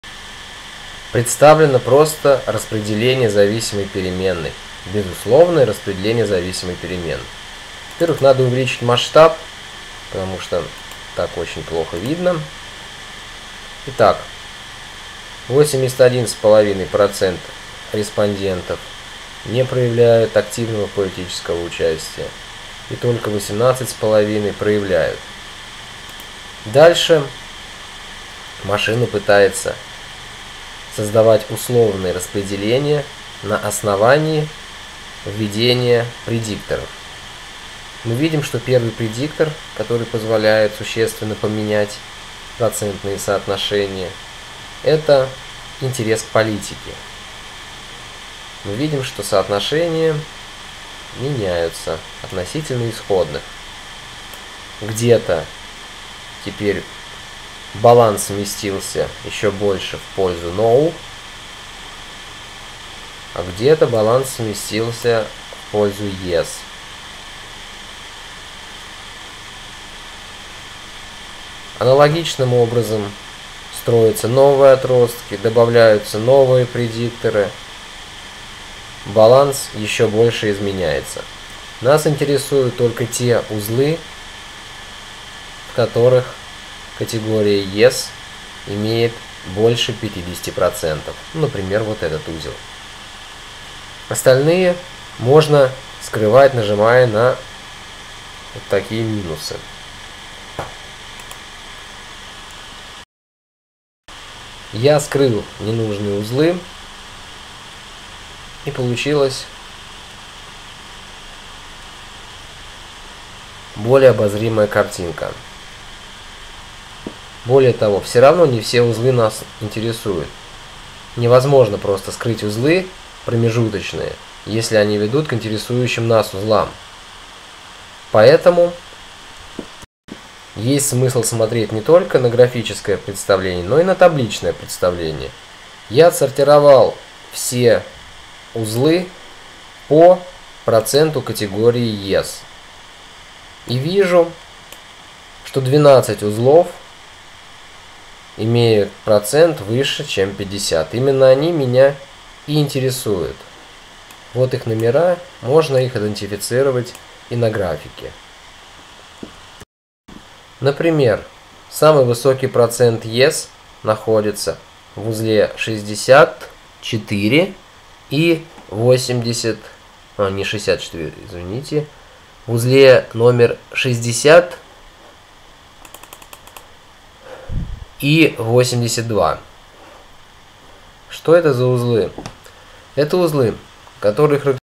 Exhaustive ChAID 7 Деревья классификации
Графическое и табличное представление модели Exhаustive ChAID.
Навигация по фрагментам видео (после перехода используйте область навигации по странице справа).